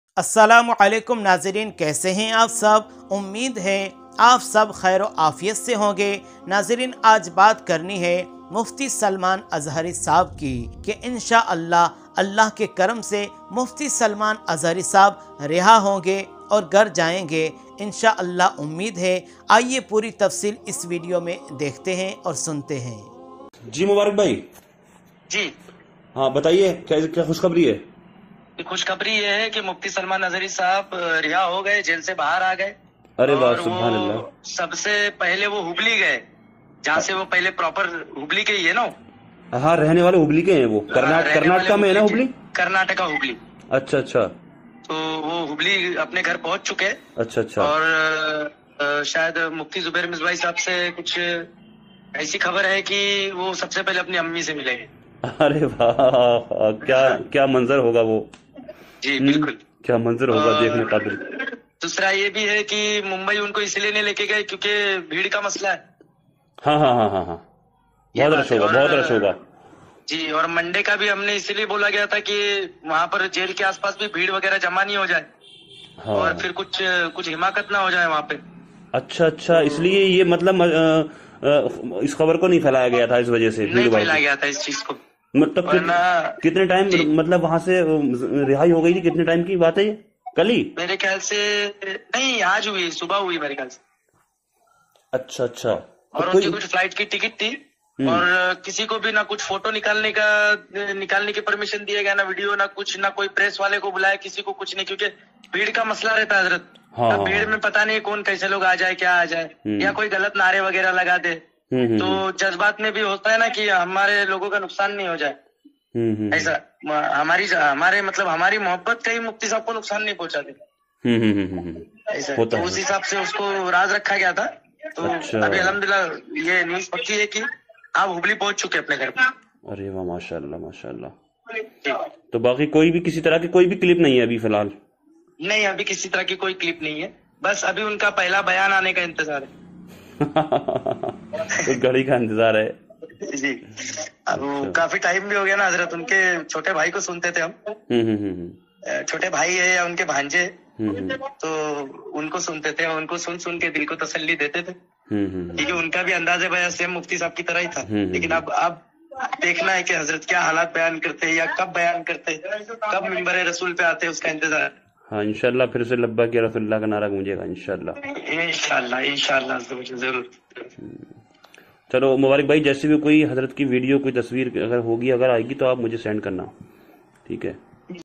Mufti Salman Azhari Ponche Apne Ghar Masha Allah | सबसे से पहले मां से मुलाकात
Assalamu Alaikum Nazreen ! Aap Ka NoorZain Knowledge YouTube Channel Par Dil Se Welcome Hai.

Is Video Me Aap Ko Mufti Salam Azhari ki new update milegi...
Puri Details Video Me Dekhen...

About Video: Mufti Salman Azhari Ka Kya Huwa ? सलमान अजहरी ऐक बहुत बड़ी खबर